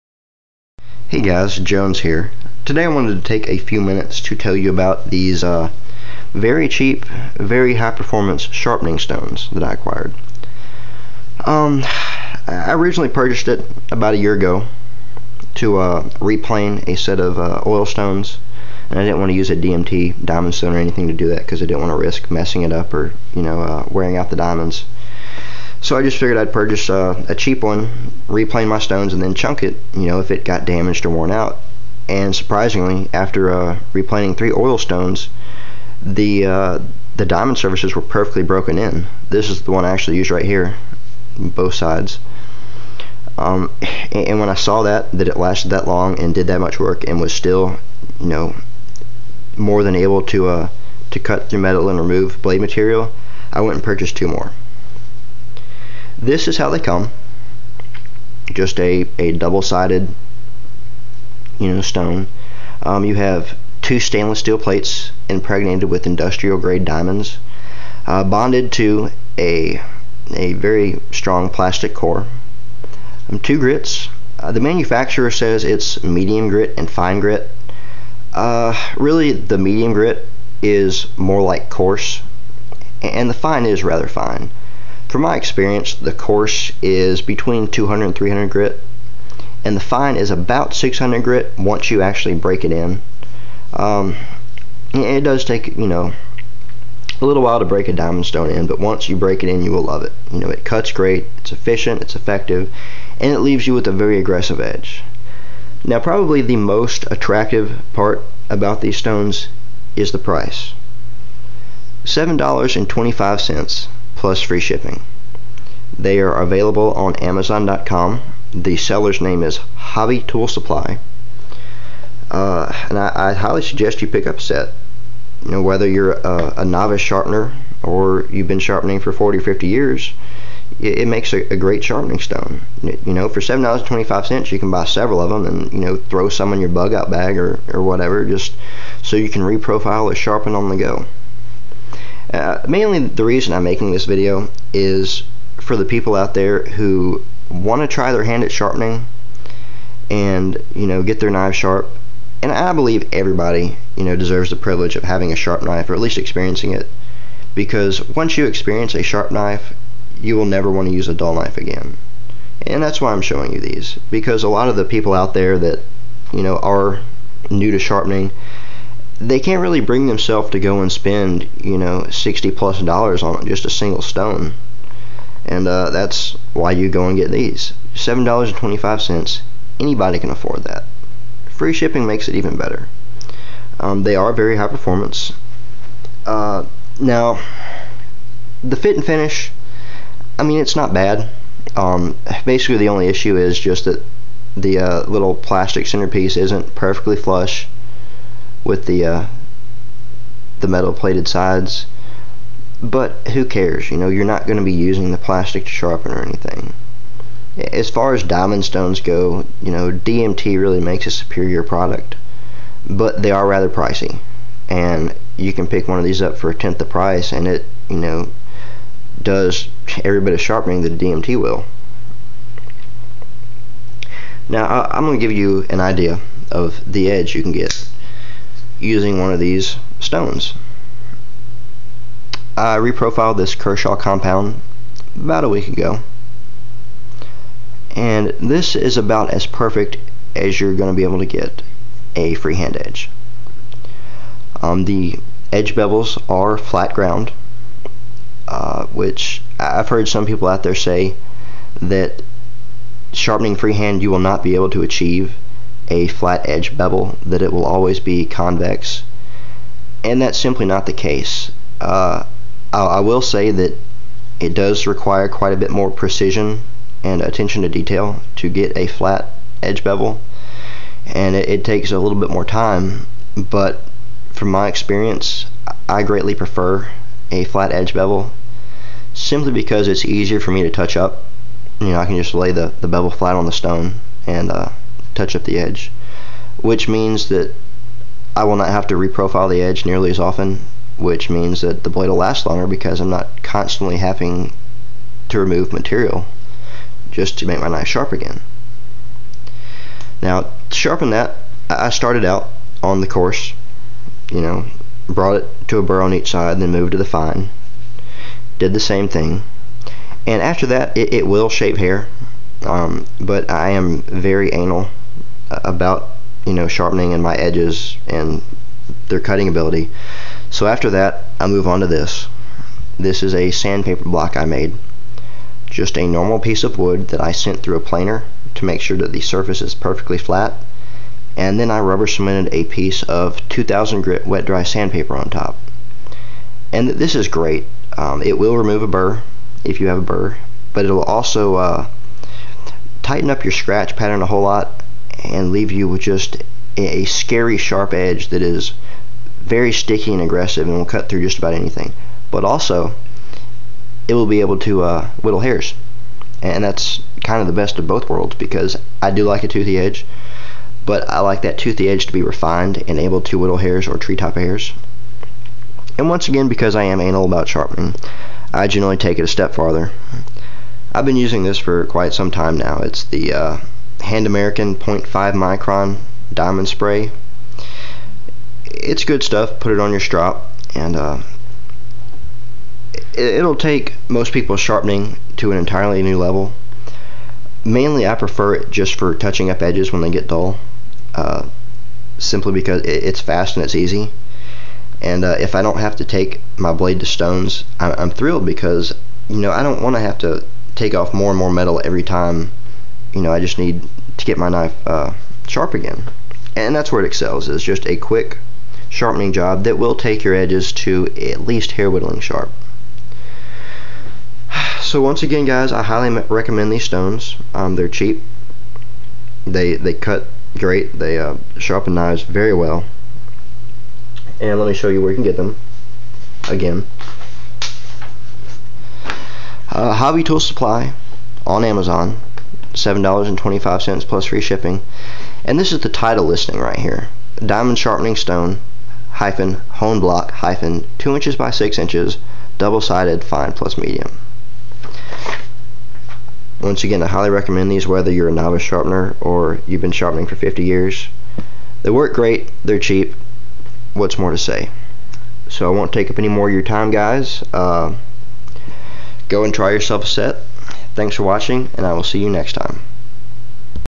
Cheap, High Performance Diamond Sharpening Stone
2" x 6" two sided diamond sharpening stone. Course (200-300 grit) and fine (~600 grit). Dirt cheap at around $7, decent build quality, and excellent performance, especially for the price. Mine have lasted a year, have sharpened hundreds of blades, and are still going strong with many years of life left in them. A great alternative to the much more expensive diamond stones.